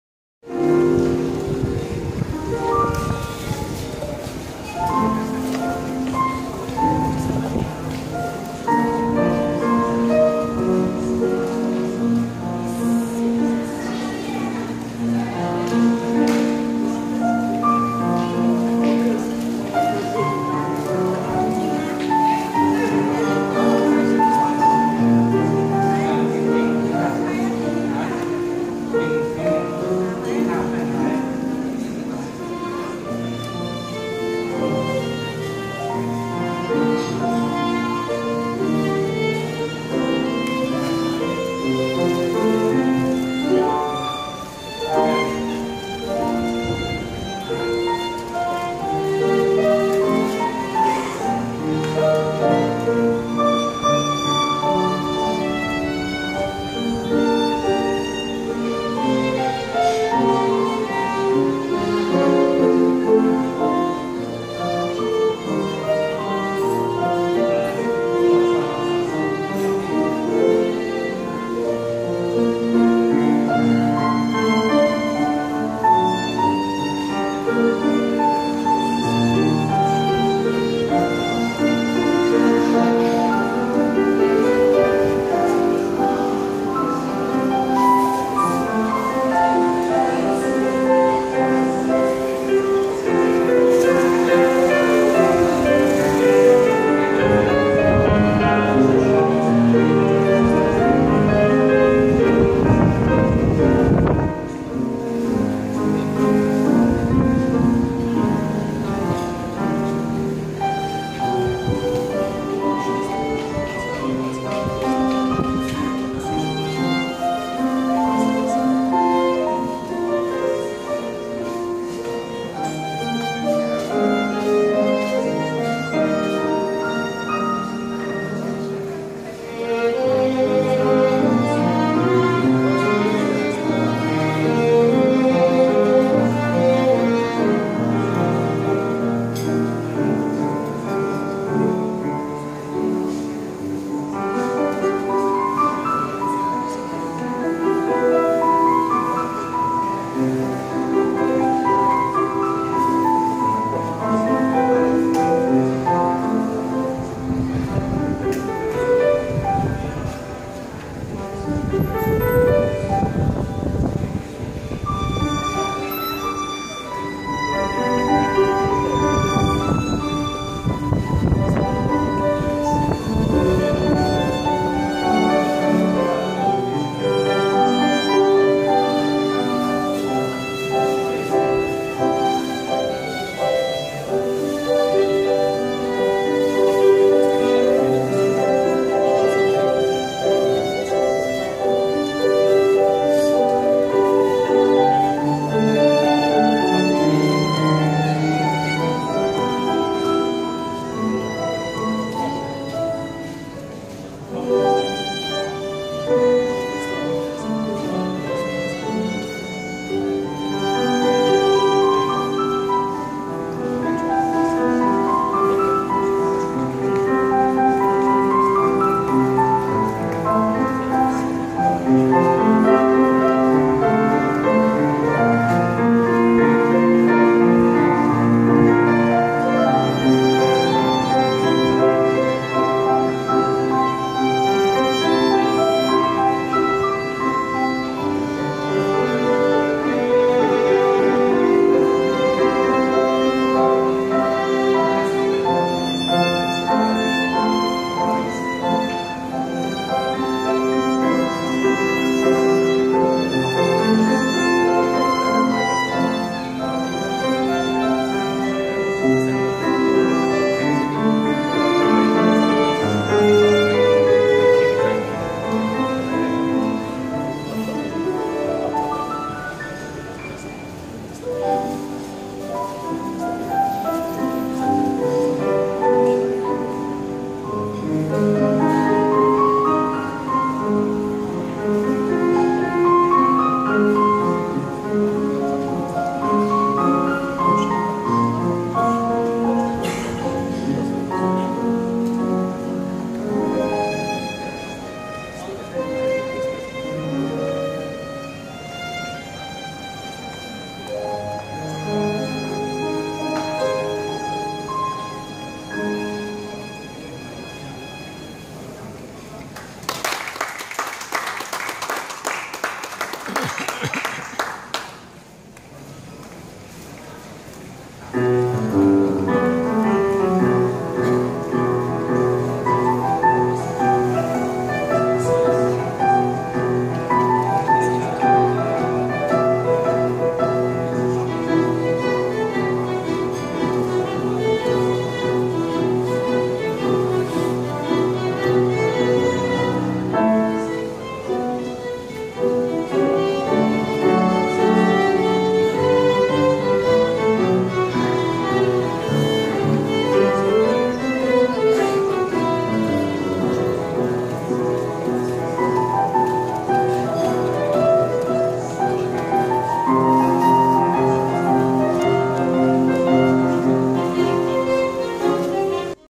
Mother and Son played Chrismas Songs 母子乐队
Mother and son done very well to entertained patients staffs and visitors at lobby B Kent Ride Wing L1.NUH visitors atS with modified Chrismas Songs.Applauded by all.His father was sitting to enjoy .A well organised family...Videoed assembled and Uploaded by Peck koon Chiong.锺伯群。 Via Samsung Note 3. At 1.30pm level L2 National University of Singapore Hospital Kent Ridge Wing Lobby B.on 17th Dec 2015 Thursday.lunch tome next to the Pharmacy..live cast.母子伴奏 圣诞节 音乐 给 国大 医院 路过者 欣赏。好评哦。可嘉